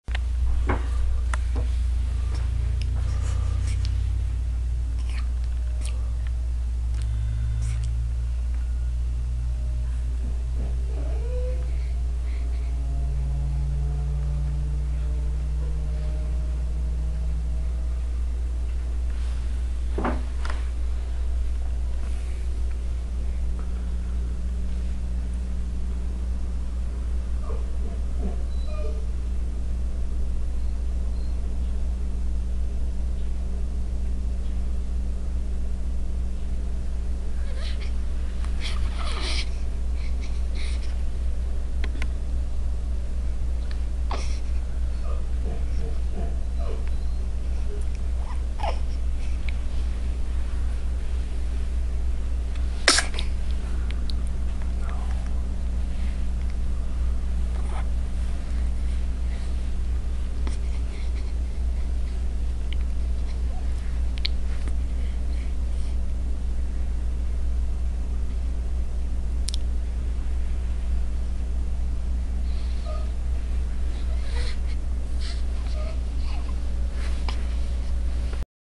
Chloe's First Day Home 10/27/11
Chloe just got home and is checking out her nursery for the first time.